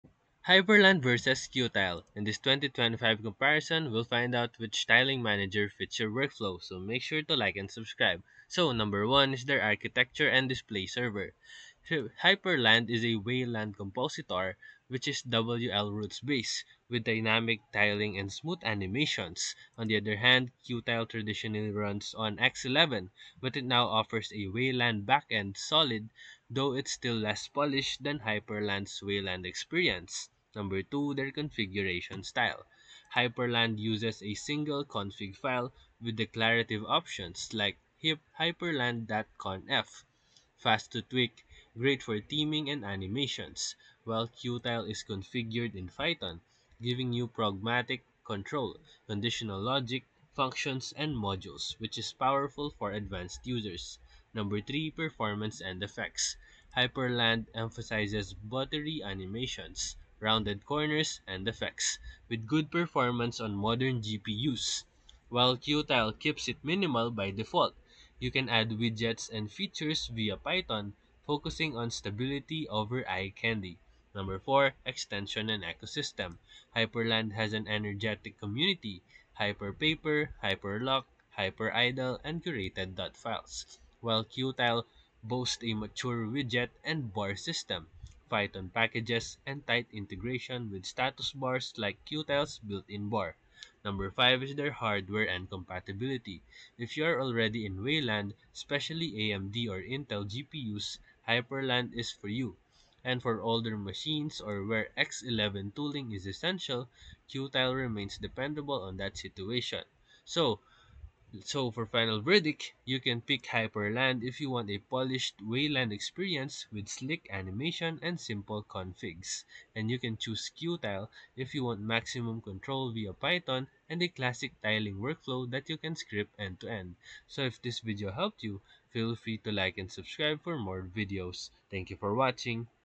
Hyprland vs Qtile 2025 – Which Is the BEST Tiling Window Manager to Use Right Now?
In this 2025 showdown, we compare Hyprland and Qtile to help you choose the best tiling window manager for your Linux setup. We’ll break down performance, customization, workflow efficiency, and ease of configuration so you can decide which one matches your style. Whether you prefer Hyprland’s sleek Wayland approach or Qtile’s Python-powered flexibility, this guide covers it all.